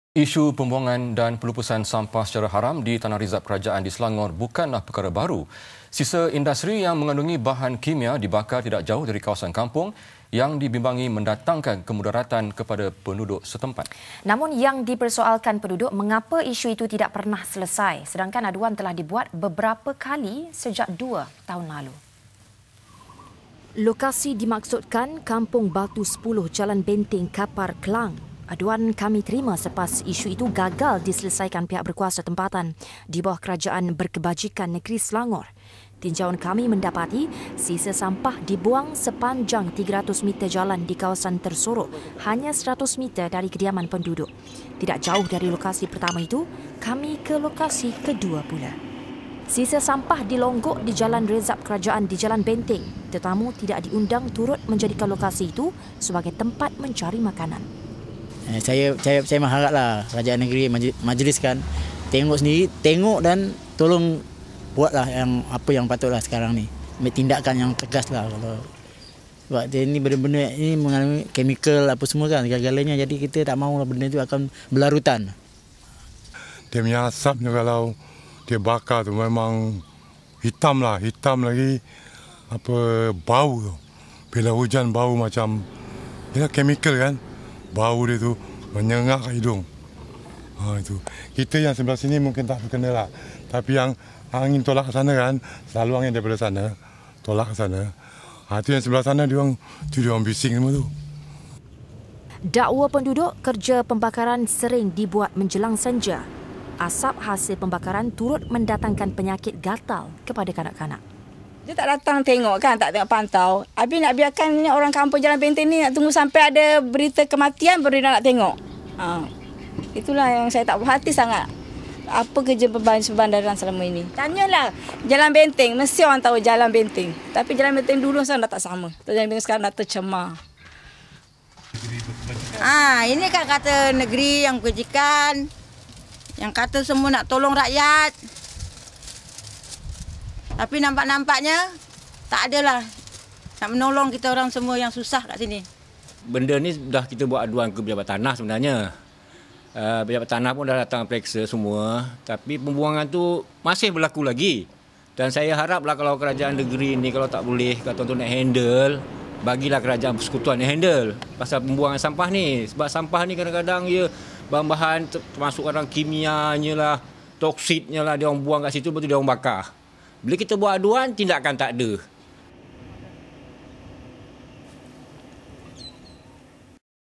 Pembuangan Dan Pembakaran Sampah Secara Haram Berleluasa Di Selangor
Isu pembuangan dan pelupusan sampah secara haram di tanah rezab kerajaan di Selangor bukanlah perkara baharu.

Sisa industri yang mengandungi bahan kimia dibakar tidak jauh dari kawasan kampung yang dibimbangi mendatangkan kemudaratan kepada penduduk setempat.

Namun yang dipersoalkan penduduk, mengapa isu itu tidak pernah selesai sedangkan aduan telah dibuat beberapa kali sejak dua tahun lalu.